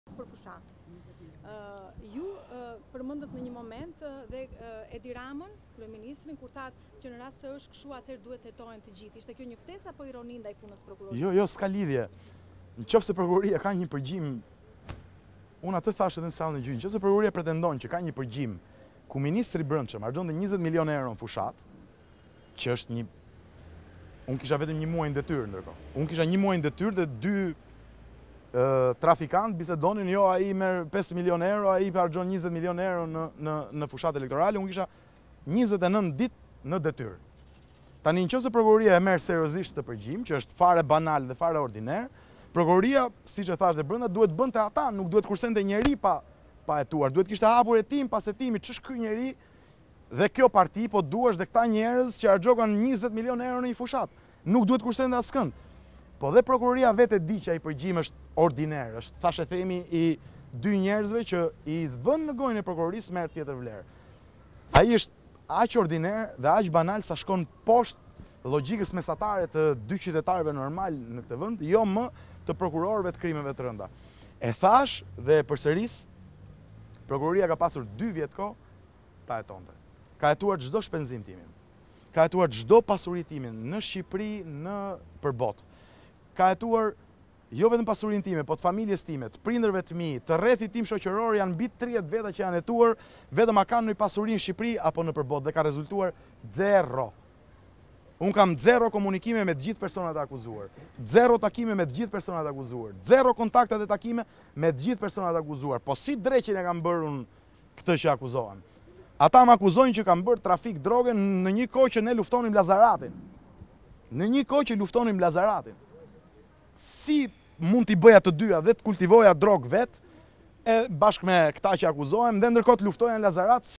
Report TV -Të hetohet edhe Rama?!/ Ja si përgjigjet Tahiri
Duke u ndalur tek përgjimet, ku dëgjohet se fitonte 5 milion euro në muaj dhe se ka shpenzuar 20 mln euro në fushatë, ish-ministri Tahiri tha se ky është një përgjim banal dhe nëse do ishte e vërtetë, nuk duhej të merrej në hetim vetëm ai, por edhe përfaqësues të tjerë të kësaj partie përfshirë edhe Ramën.

"Shpenzimet për fushatë, 5 milionë euro në një muaj: Të hetohet edhe Rama?! Nëse prokuroria ka një përgjim ku ministri i brendshëm harxhon 20 milionë euro në fushatë, unë kisha vetëm një muaj në detyrë dhe dy trafikantë bisedonin ai merr 5 milionë euro në muaj, nëse prokuroria e merr seriozisht këtë përgjim banal, nuk duhej të kursente njeri pa hetuar. Cështë kjo parti dhe këta njerëz që harxhokan 20 milionë euro në fushatë? Por edhe prokuroria e di që ai përgjim është banal dhe ordiner. Prokuroria ka pasur dy vjet kohë ta hetonte. Ka hetuar cdo pasuri timen, në Shqipëri, nëpër botë, pasurinë e prindërve të mi, të rrethit tim shoqëror, mbi 30 persona. Unë kam zero kontakte dhe takime me personat e akuzuar. Si dreqin e kam bërë unë këtë", u përgjigj Tahiri pyetjeve të gazetarëve.